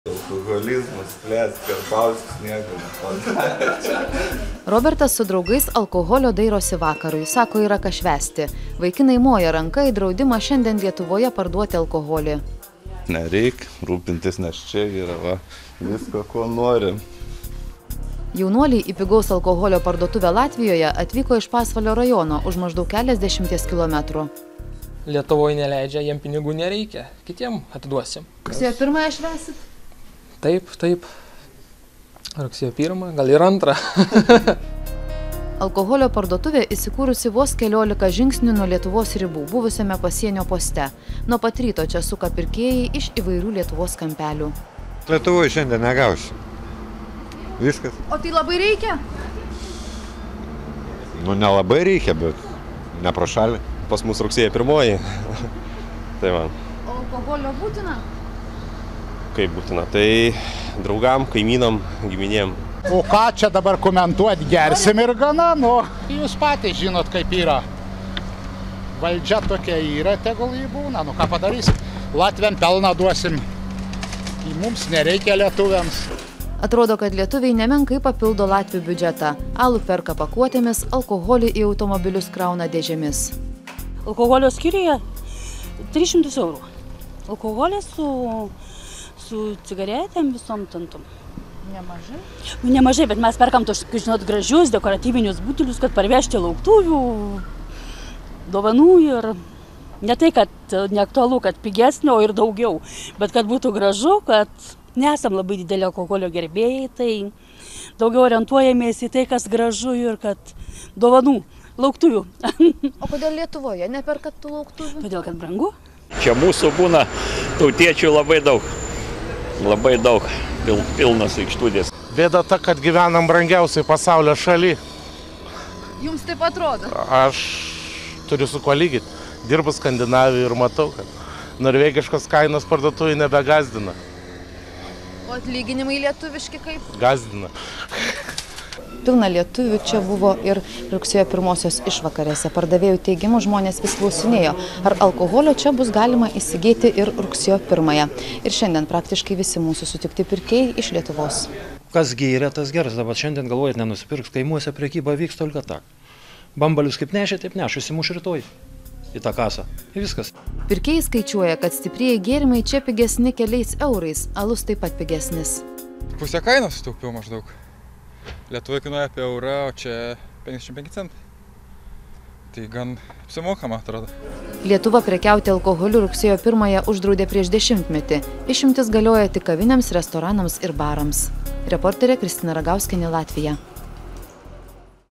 Lietuviai rugsėjo pirmąją pildo brolių latvių biudžetą
Lietuva mini dešimtmetį, kai parduotuvėse rugsėjo pirmąją uždrausta prekiauti alkoholiu. Tačiau akivaizdu, kad lietuviams tai tikrai ne blaivybės diena. Nuo pat ryto tautiečiai alkoholį dėžėmis tempia iš Latvijos. Lietuviai skaičiuoja čia paliekantys šimtus eurų.